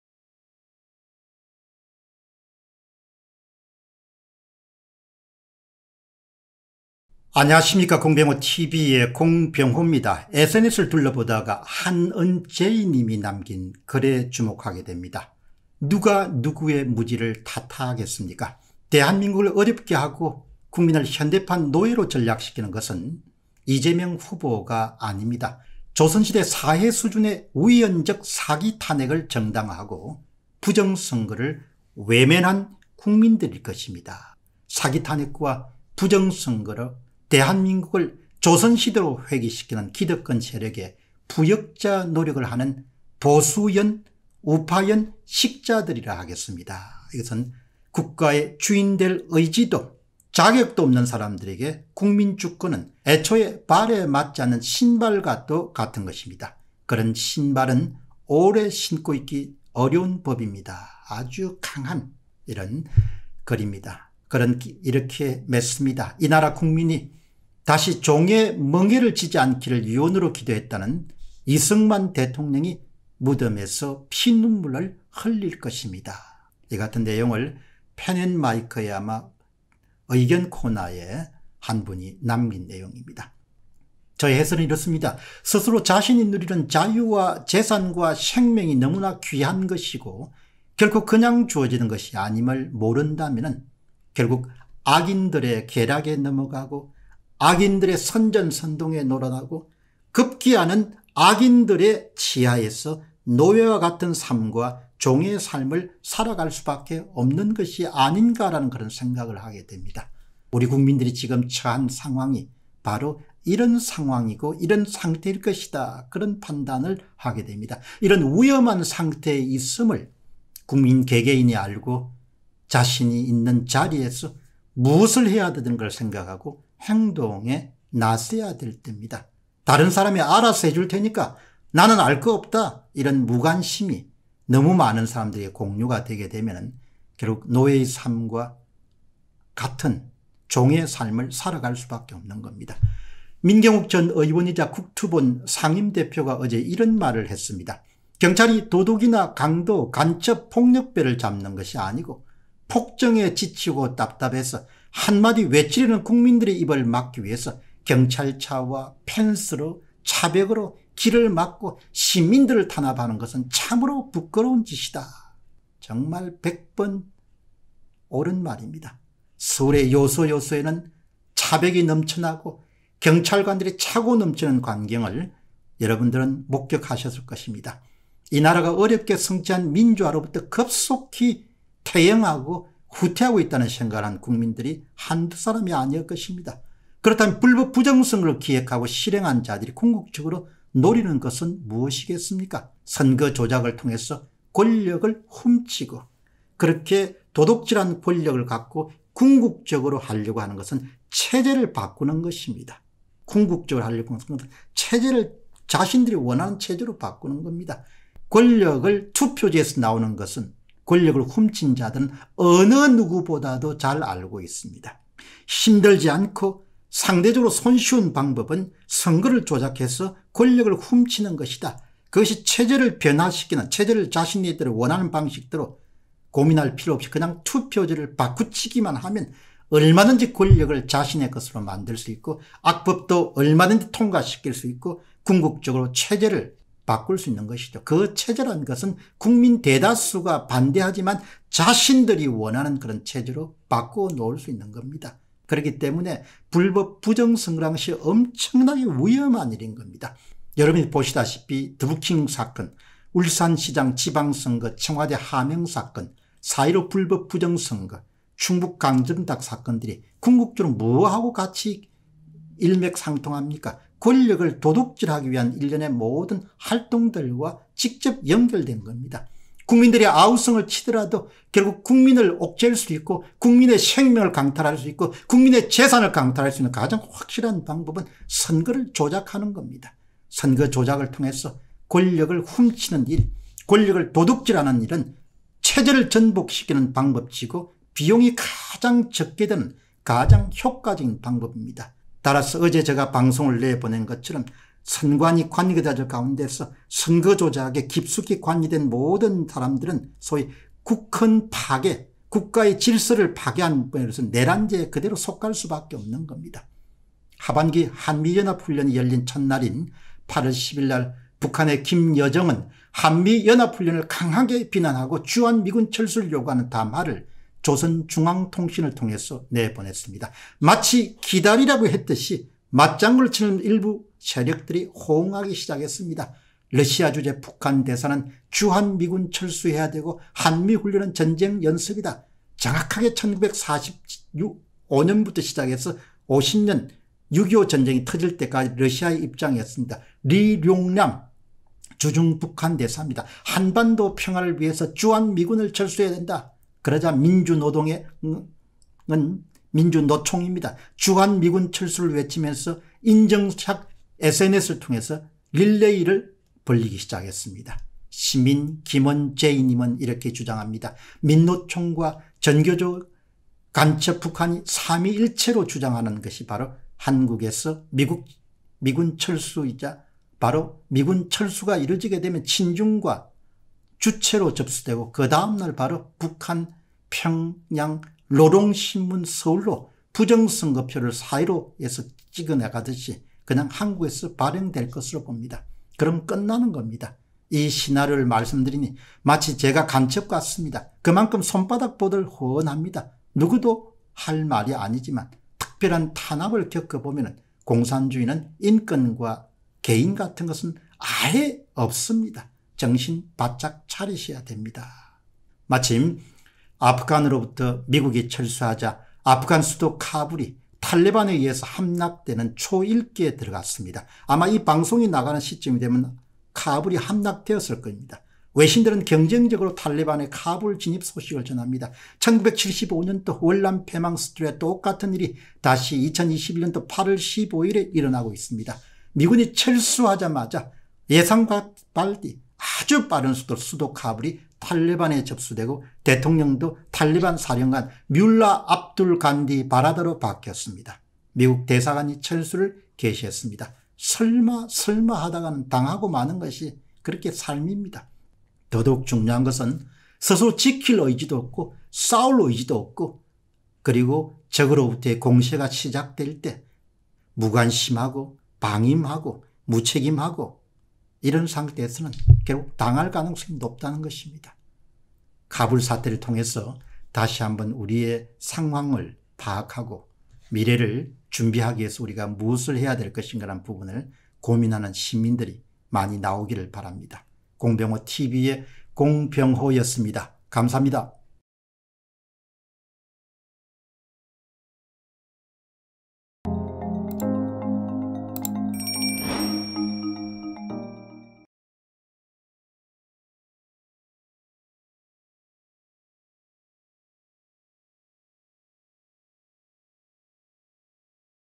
아프간 수도, 카불 함락 / 설마설마 하다가는 / 미련한 자들은 꼭 겪어봐야 알고 / 현명한 자들은 모든 일을 반면교사로 삼아서 안다 / 지킬 의지가 없다면 [공병호TV]
Bitcoin : 3MTW849AkP2ftLeSK2teZHMmzLu7Z83s9U